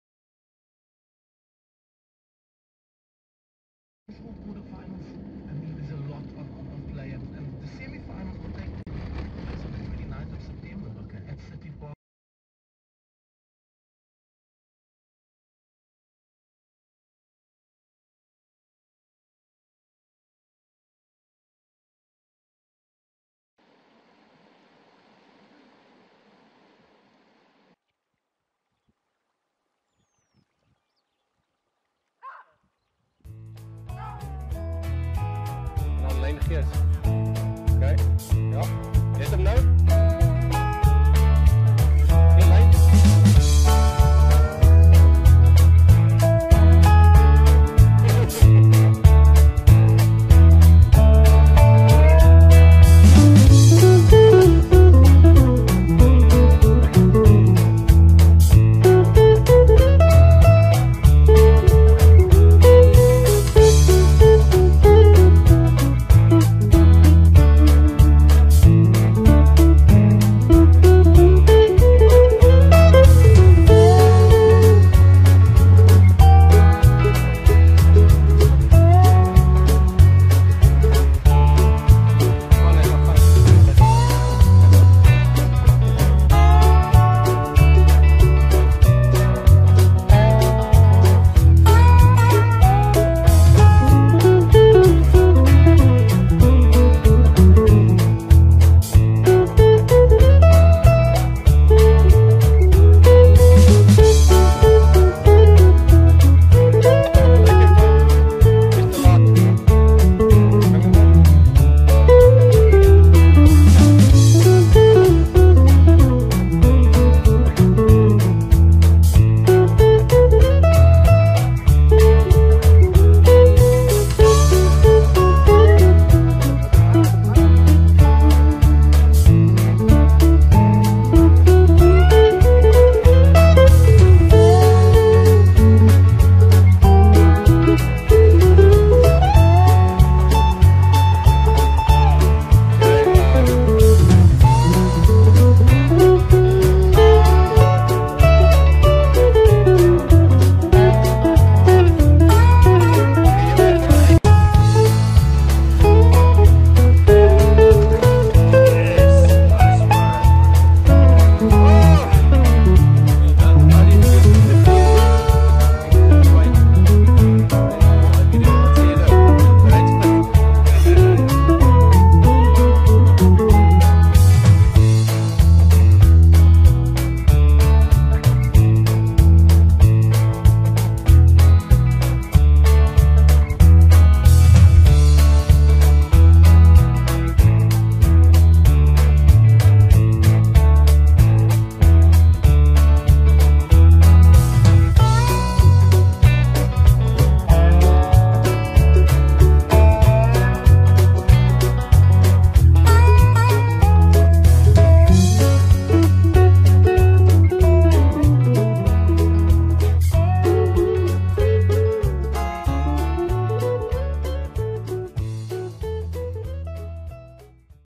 Linecasters - Kalahari Flyfishing Safari
Watch Linecasters tackle the yellow gold of the lower Orange River during a recent fly fishing safari in the Kalahari.